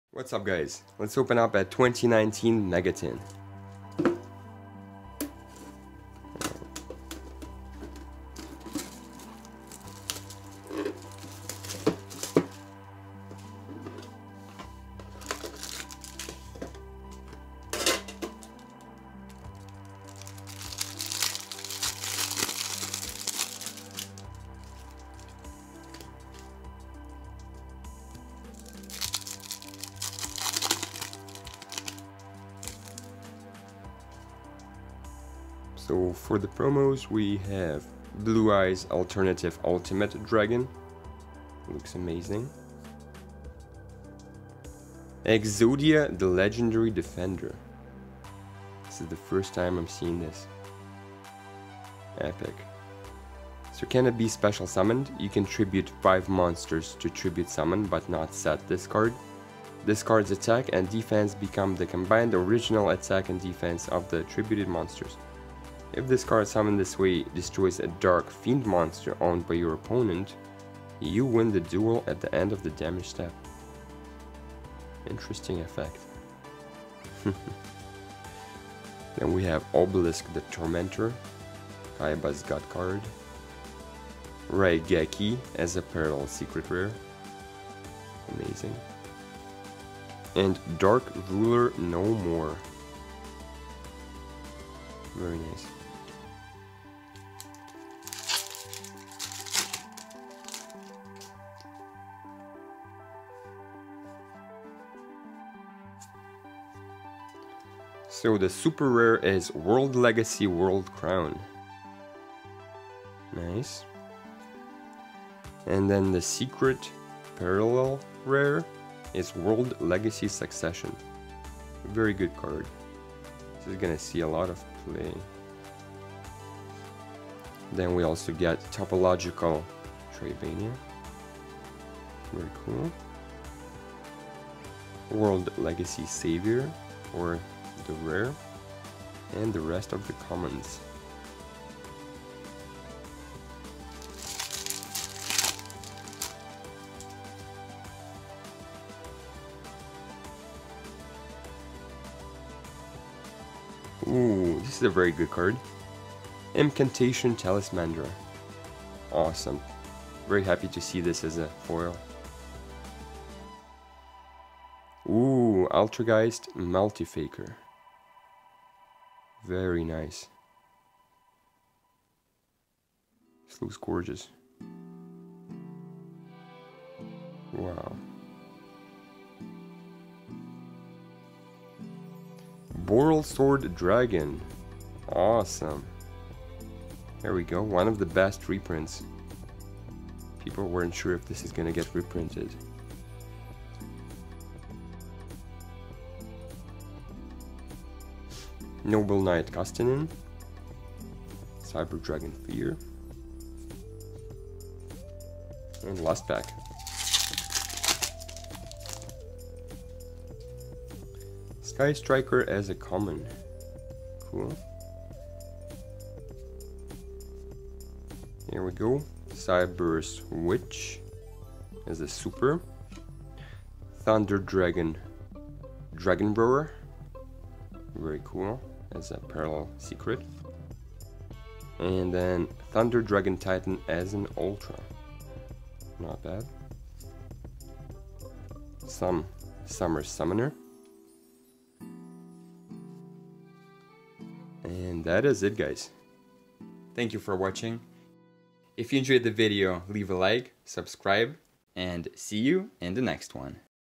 YuGiOh 2019 Gold Sarcophagus Mega Tin Opening # 5
We open a Gold Sarcophagus 2019 Mega-Tin in 4K!

Each Mega Tin contains 5 Prismatic Secret Rare cards:
2 of 6 new cards with art by Kazuki Takahashi.
2 of 5 cards from the original Yu-Gi-Oh! animated series.
1 of 3 new World Premiere cards.
and 3 Gold Sarcophagus Tin 2019 Mega Packs

The Mega Packs include reprints from the following sets:
Flames of Destruction, Dark Saviors, Cybernetic Horizon, and Soul Fusion. Notable reprints: "Raigeki", "Monster Reborn", "Altergeist Multifaker", "Vampire Sucker", "Knightmare Unicorn", "Knightmare Mermaid", "Called by the Grave", "Borrelsword Dragon", "Cyber Dragon Sieger", "Danger! Nessie!", "Danger!? Jackalope?", "Thunder Dragon Colossus", "Trap Trick", "Danger!? Tsuchinoko?", "Beat, Bladesman Fur Hire", "Sky Striker Ace - Kagari", "Sky Striker Mobilize - Engage!", "Sky Striker Mecha - Widow Anchor", and many more.

Each 16-card Mega-Pack has 12 Commons,1 Rare, 1 Super Rare, 1 Ultra Rare, and 1 Prismatic Secret Rare. For the first time ever all of the foil cards in these Mega-Packs appear in different rarities from their original 2018 printings.